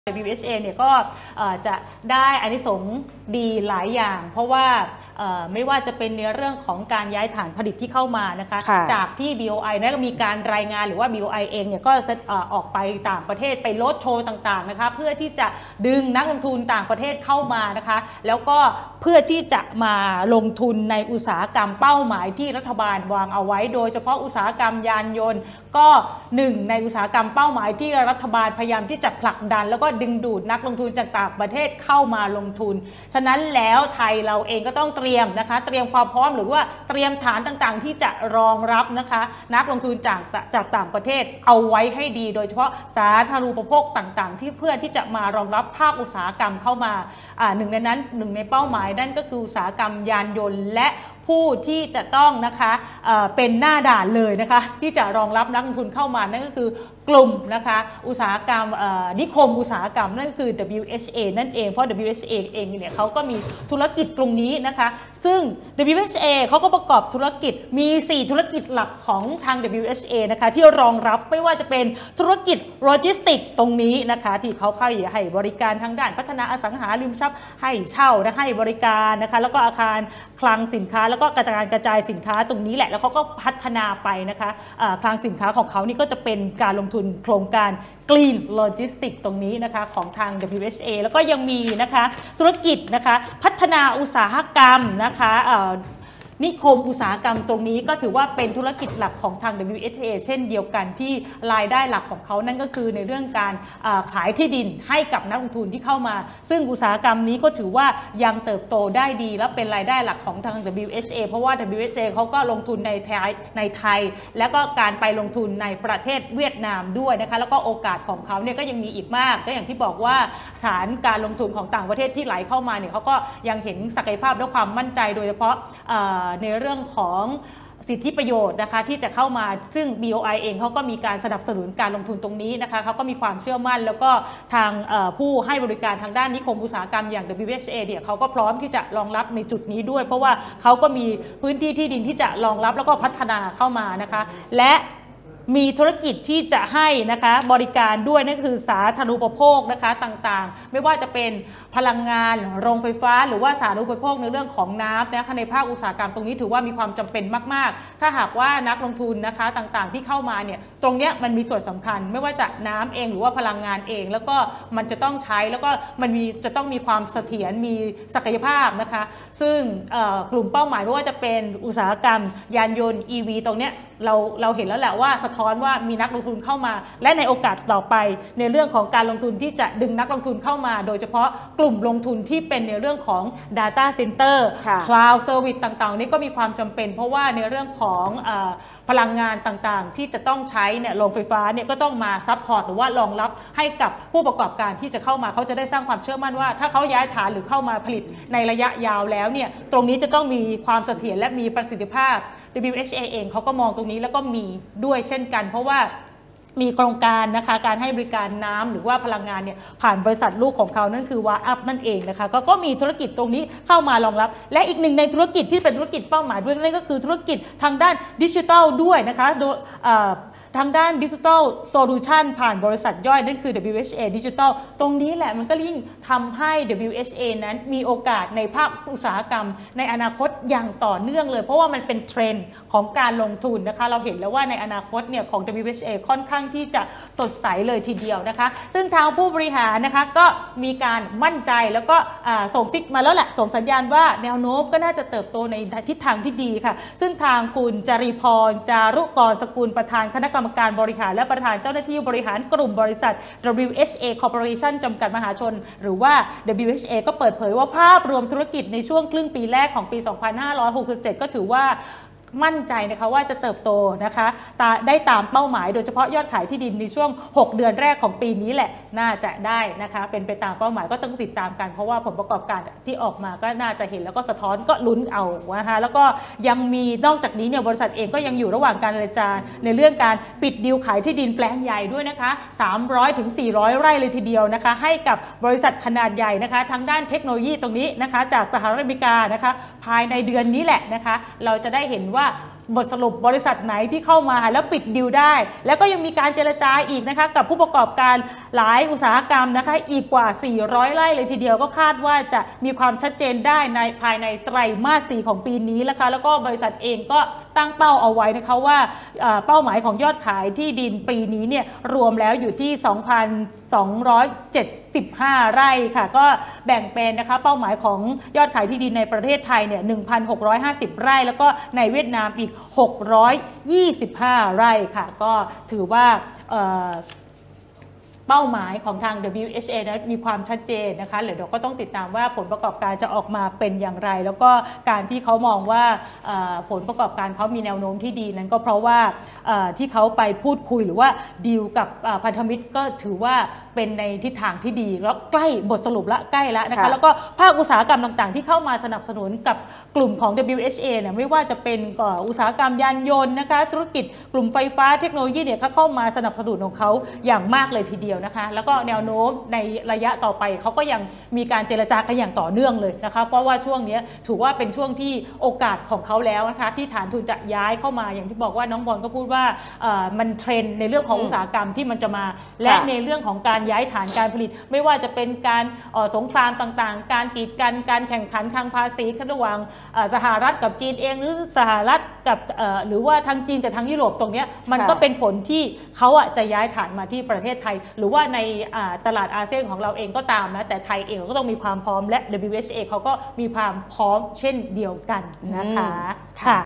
ประเด็น HOT : "WHA โอกาสทองย้ายฐาน เร่งปิดดีลขายที่ดิน" 🎯
ประเด็น HOT :  "WHA โอกาสทองย้ายฐาน เร่งปิดดีลขายที่ดิน"/ ทันหุ้นทันเกม / 9  สิงหาคม  2567🎯

รู้ทันเกม  รู้ก่อนใคร  ติดตาม  "ทันหุ้น"  ได้ทุกช่องทางเหล่านี้